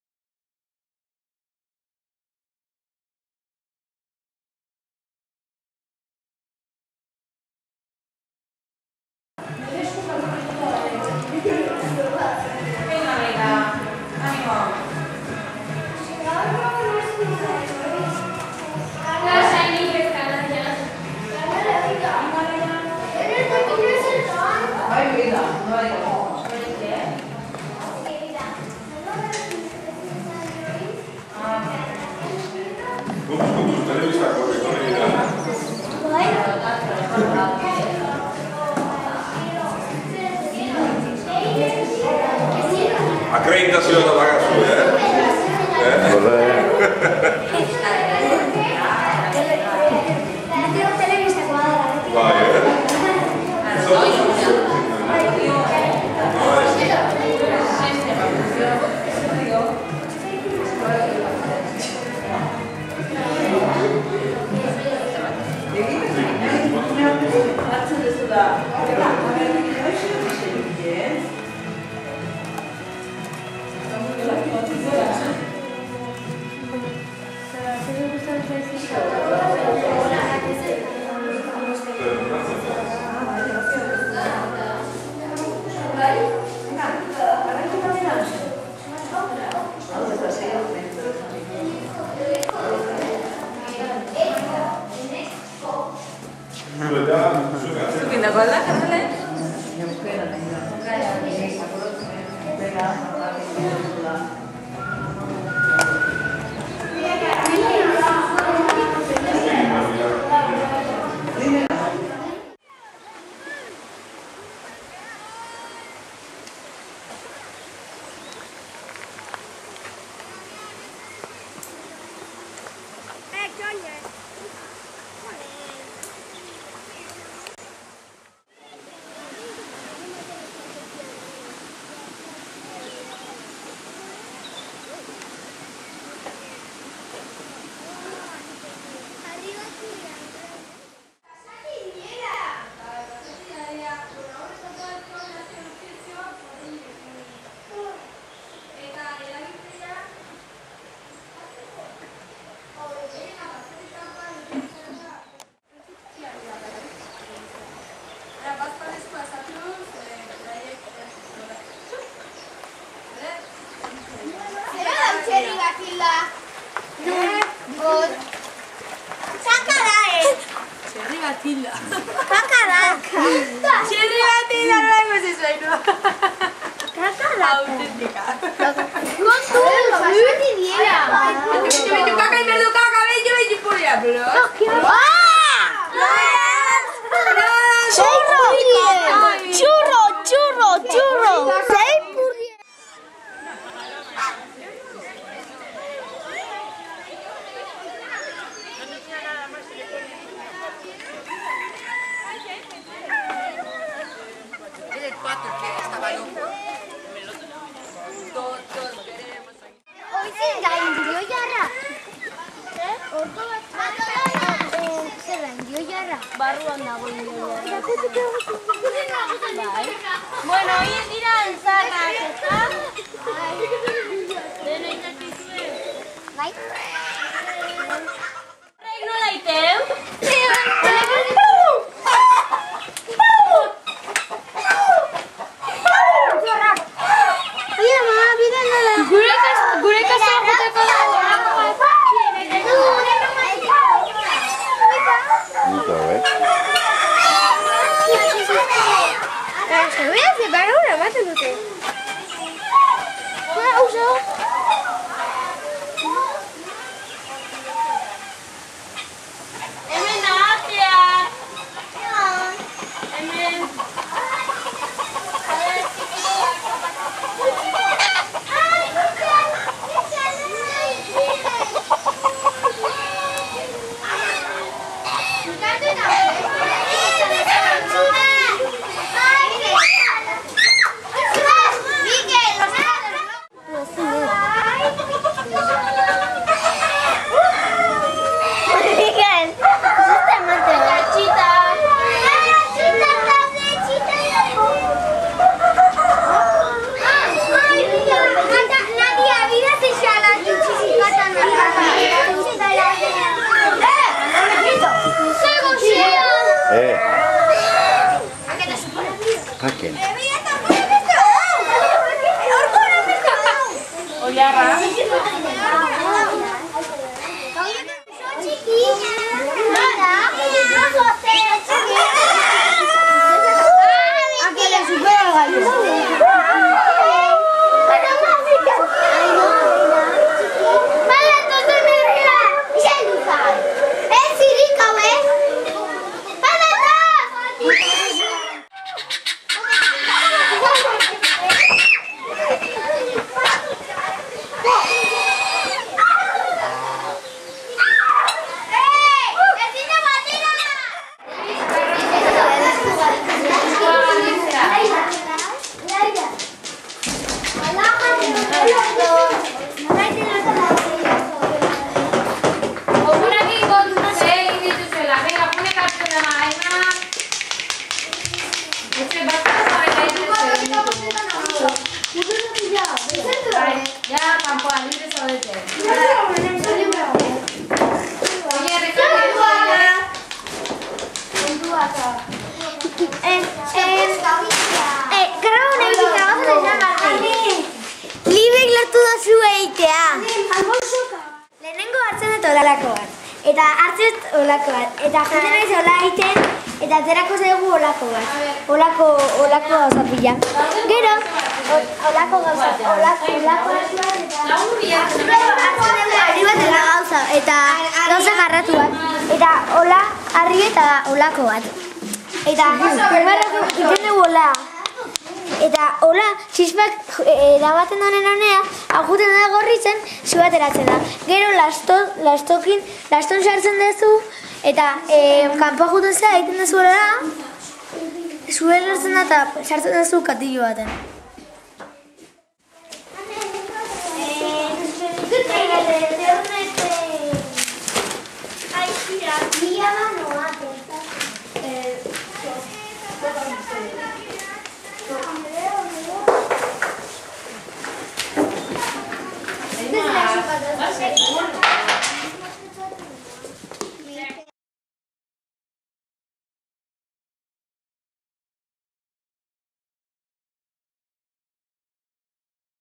Sastarrain 2011 goiza - Jakintza Ikastola
2. eguneko goiza Sastarrainen. Batzuk saskigintzan, besteak zalditegian, besteak Ekain kobazuloan...